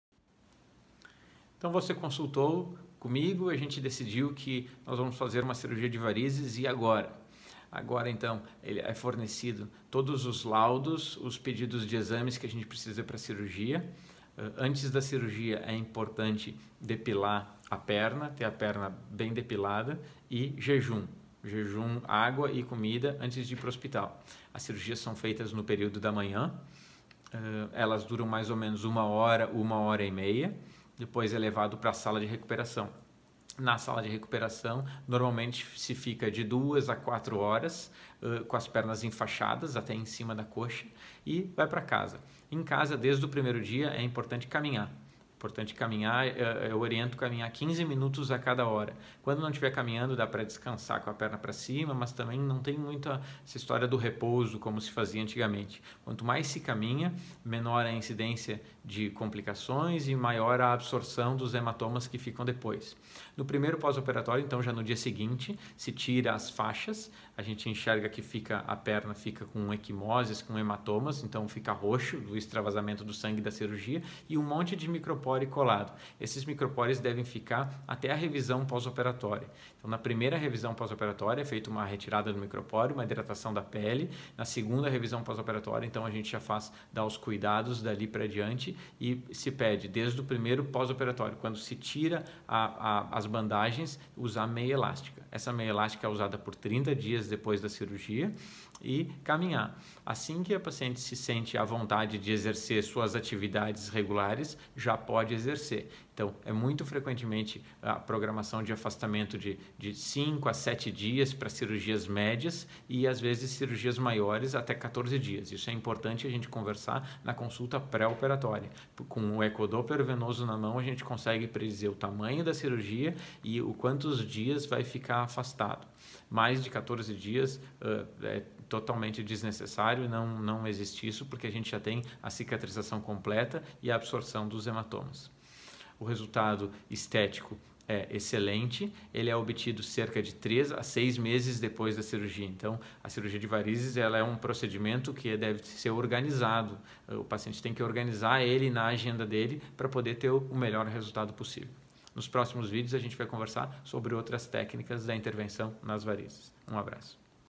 Vou fazer uma cirurgia de varizes. E agora? (varizes // Pós-operatório)
Será necessário operar suas varizes. E agora? O que fazer? Como funciona?
Nesse vídeo, o Dr. Vinicius Lain explica detalhadamente o pré-operatório, o dia da cirurgia e como é a recuperação em casa para pacientes que operam as varizes.
A cirurgia de varizes tem um excelente resultado estético e o retorno às atividades normais se dá de 5 a 7 dias nas cirurgias médias e até 14 dias nas cirurgias maiores.